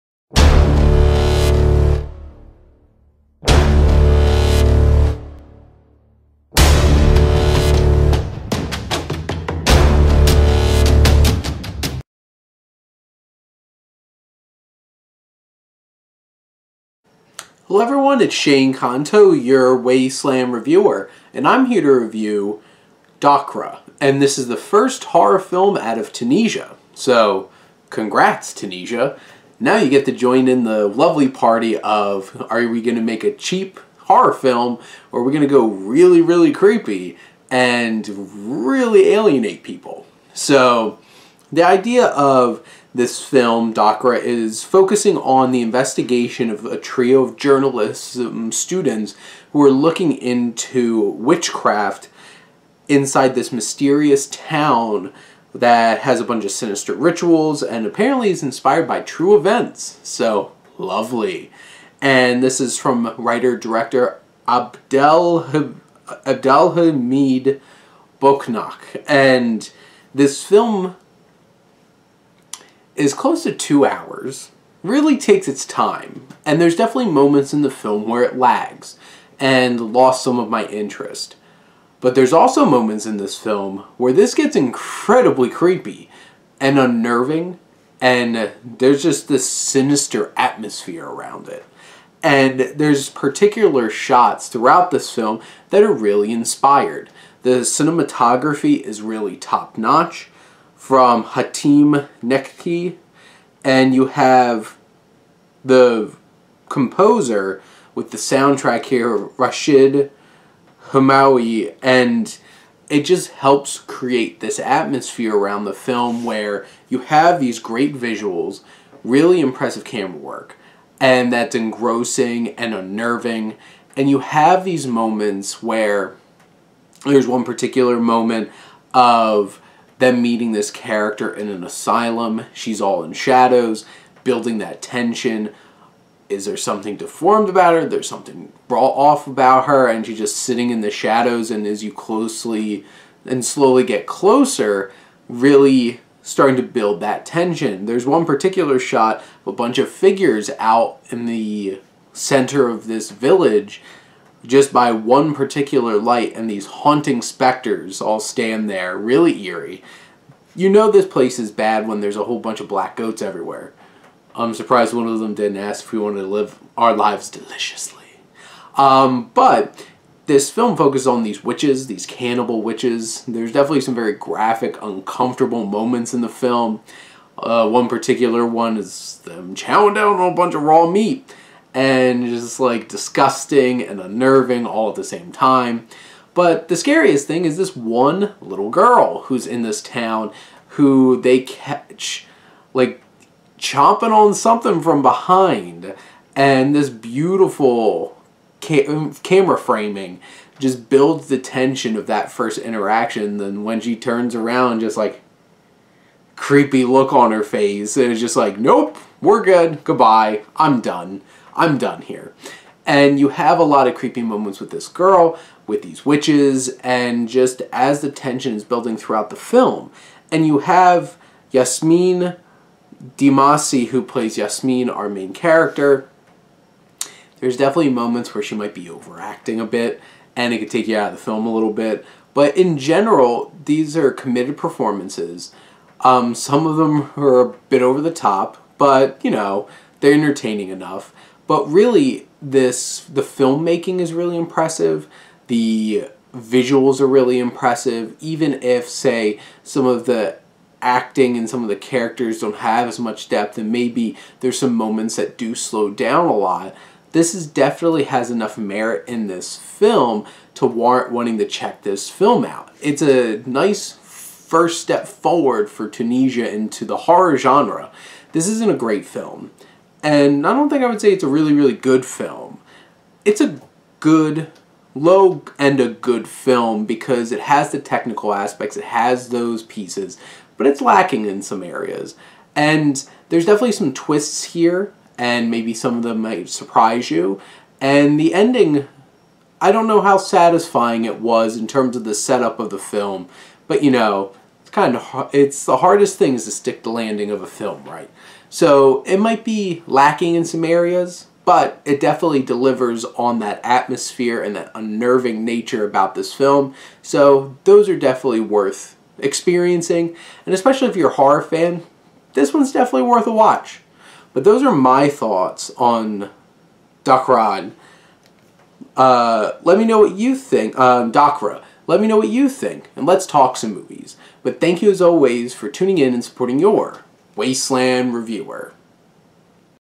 Dachra (2021) - Wasteland Review
My Wasteland Review of Dachra
Directed by Abdelhamid Bouchnak
Starring: Yassmine Dimassi, Hela Ayed, Aziz Jebali
Available to rent of VOD
Synopsis:
An investigation into witchcraft leads a trio of journalism students to a mysterious town marked by sinister rituals. Inspired by true events.